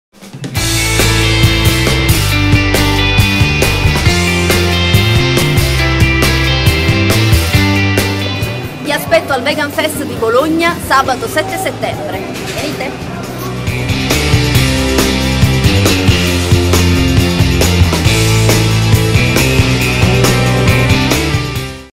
Loredana Cannata invita al VeganFest 2013
L'attrice Loredana Cannata invita i suoi fans a partecipare al VeganFest 2013 dove verrà presentata la campagna di Animalisti Italiani "VeganBeauty" che vede Loredana nel ruolo di testimonial.